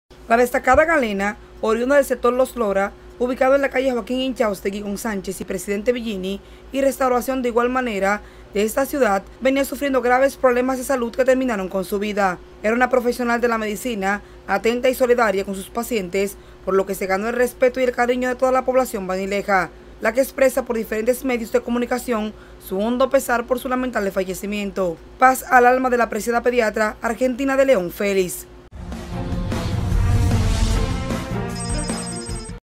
Causa consternación muerte de pediatra Argentina De León Félix.
Consternación ha causado en esta ciudad Baní, la muerte de la reconocida pediatra, Argentina De León Félix.